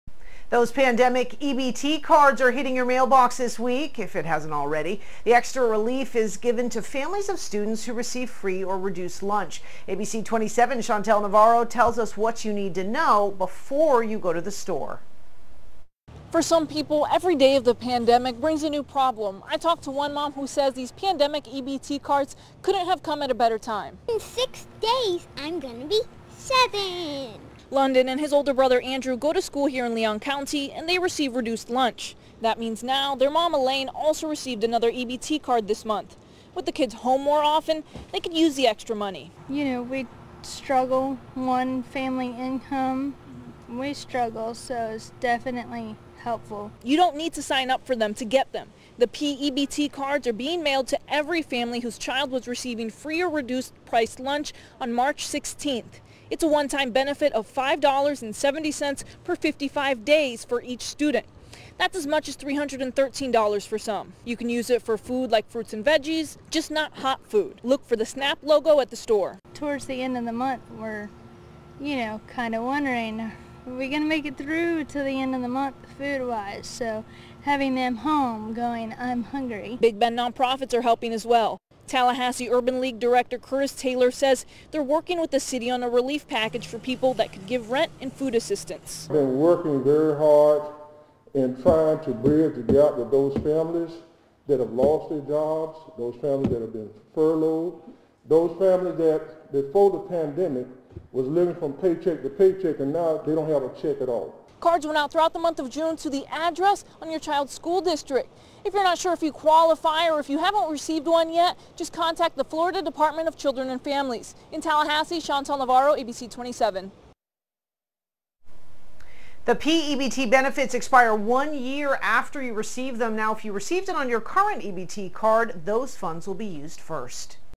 Pandemic-EBT cards begin arriving in mailboxes across Florida
Pandemic-EBT cards are now hitting mailboxes across Florida. But make sure to be on the lookout as the card can be easy to miss.